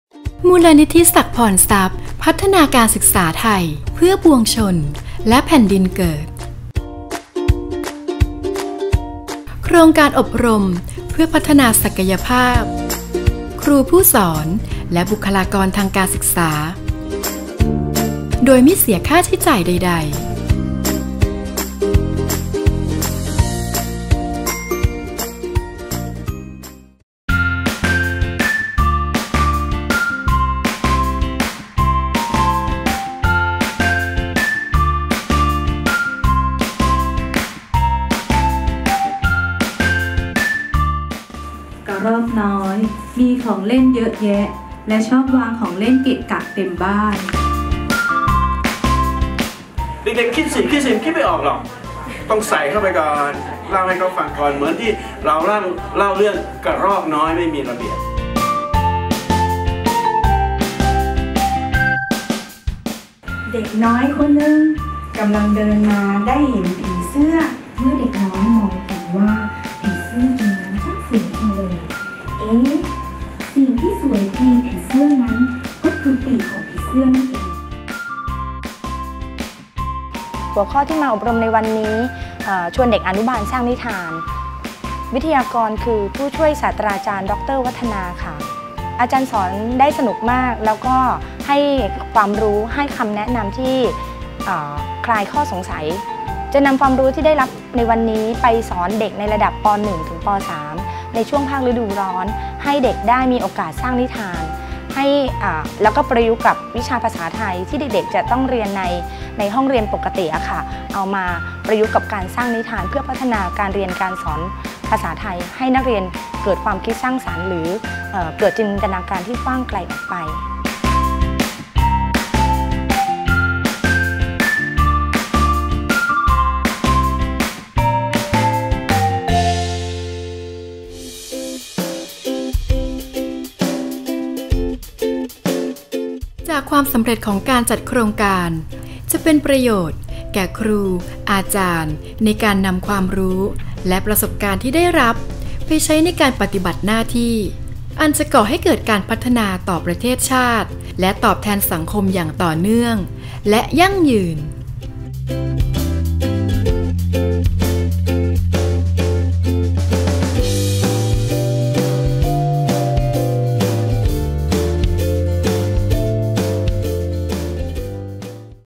หลักสูตร ชวนเด็กอนุบาลสร้างนิทาน
วิทยากร : ผศ.ดร.วัฒนา มัคคสมัน
วันที่  11 มีนาคม 2560 เวลา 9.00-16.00 น.
สถานที่อบรม : สำนักงานมูลนิธิศักดิ์พรทรัพย์